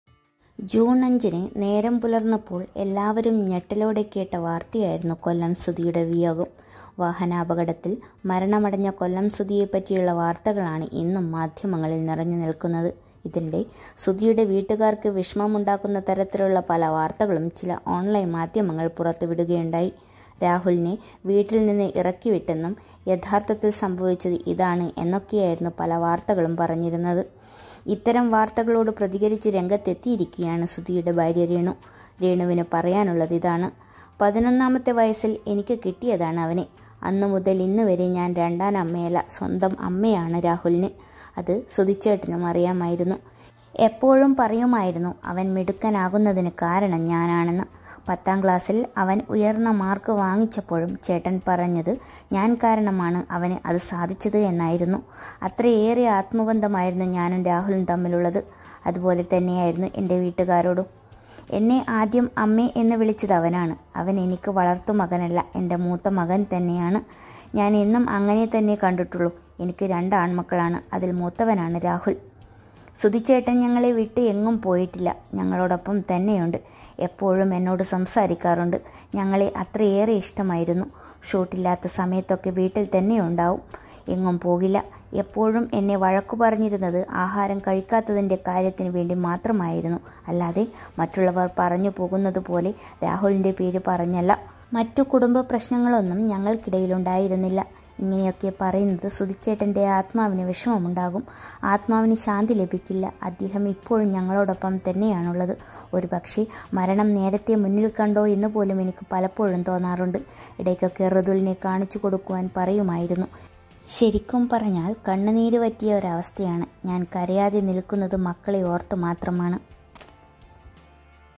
തെറ്റായ വാർത്തകൾ പ്രചരിപ്പിക്കുമ്പോൾ ഞങ്ങളെക്കാൾ ഏറെ വേദനിക്കുന്നത് സുധി ചേട്ടന്റെ ആത്മാവാണ്..
തെറ്റായ വാർത്തകൾ പ്രചരിപ്പിക്കുമ്പോൾ ഞങ്ങളെക്കാൾ ഏറെ വേദനിക്കുന്നത് സുധി ചേട്ടൻറെ ആത്മാവാണ്; നിങ്ങൾ പറയുന്നതൊന്നുമല്ല സത്യം: ഉള്ളുരുകി രേണു..
kollam sudhi family